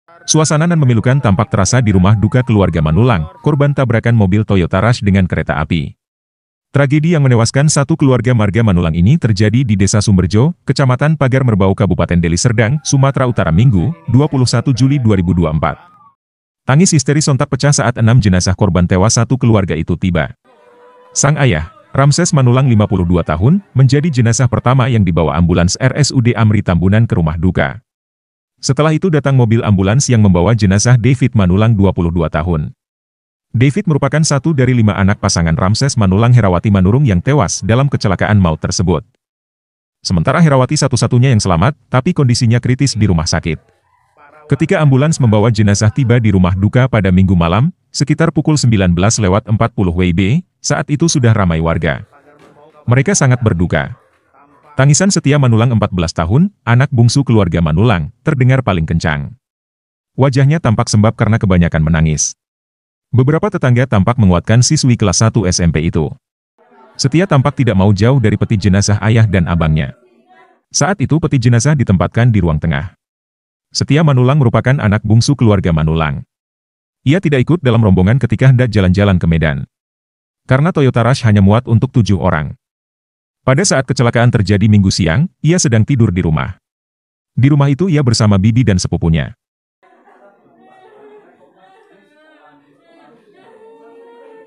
Pilu, Putri Bungsu Histeris saat 6 Jenazah Keluarga Manullang Tiba di Rumah
Suasana nan memilukan tampak terasa di rumah duka keluarga Manullang, korban tabrakan mobil Toyota Rush dengan kereta api.

Tragedi yang menewaskan satu keluarga marga Manullang ini terjadi di Desa Sumberjo, Kecamatan Pagar Merbau, Kabupaten Deli Serdang, Sumatera Utara, Minggu, 21 Juli 2024.

Tangis histeris sontak pecah saat 6 jenazah korban tewas satu keluarga itu tiba. Sang ayah, Ramses Manullang, 52 tahun, menjadi jenazah pertama yang dibawa ambulans RSUD Amri Tambunan ke rumah duka.

Setelah itu datang mobil ambulans yang membawa jenazah David Manulang, 22 tahun. David merupakan satu dari lima anak pasangan Ramses Manullang-Herawati Manurung yang tewas dalam kecelakaan maut tersebut.

Editor Video: Vlad